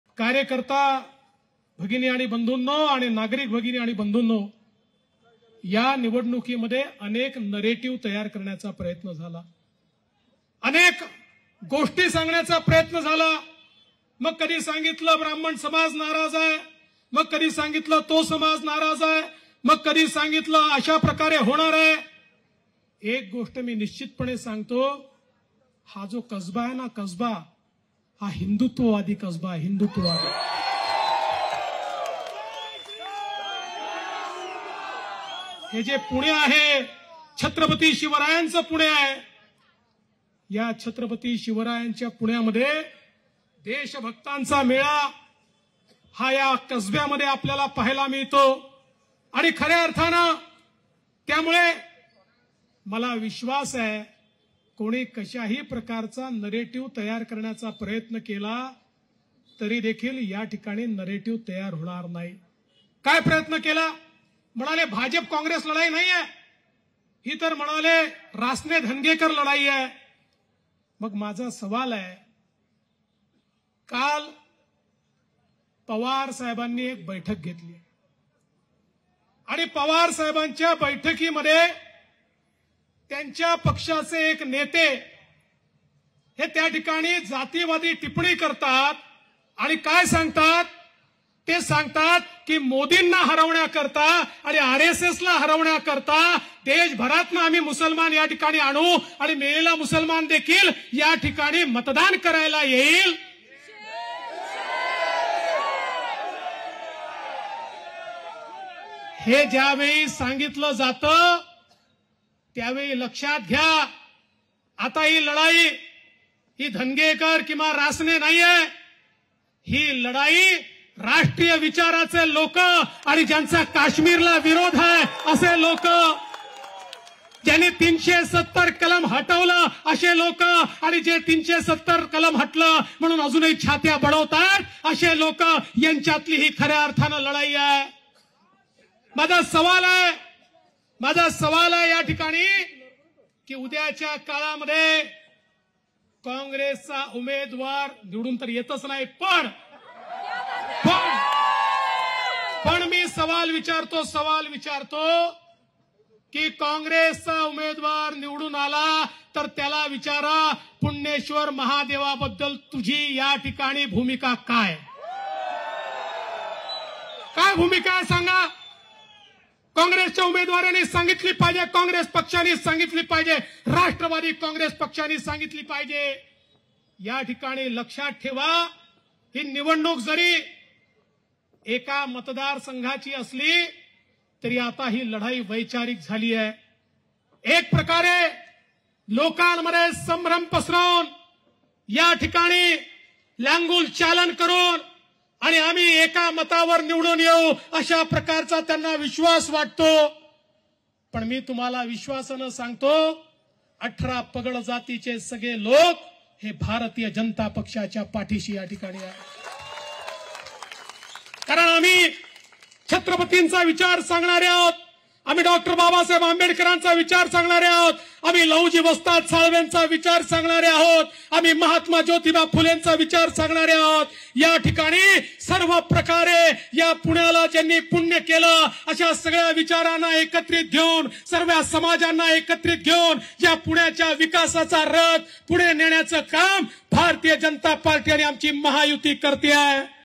कसब्याची लढाई राष्ट्रप्रेमी आणि राष्ट्रद्रोही यांच्यामध्ये! | DCM Devendra Fadnavis
(कसबा पेठ, पुणे । 23/2/23)